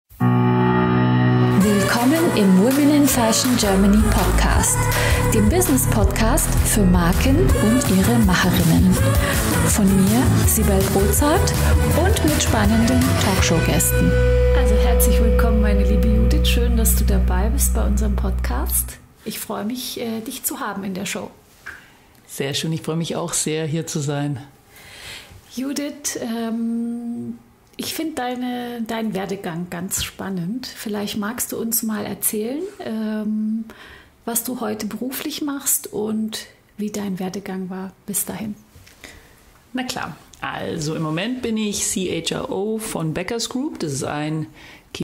(1/5) Judith Jungmann im Business-Podcast für Marken und ihre Macherinnen (Snippet 1)
Judith Jungmann (CHRO) - über die Freisetzung von Potenzialen: Definiere Deinen Purpose, um als Personal Brand erfolgreich zu sein.

Judith ist zu Gast bei mir im Business-Podcast für Marken und ihre Macherinnen.

Und hier für Dich ein exklusiver Vorgeschmack auf die vollständige Episode, die es ab dem 25.09.20 überall gibt, wo Du Podcasts hören kannst.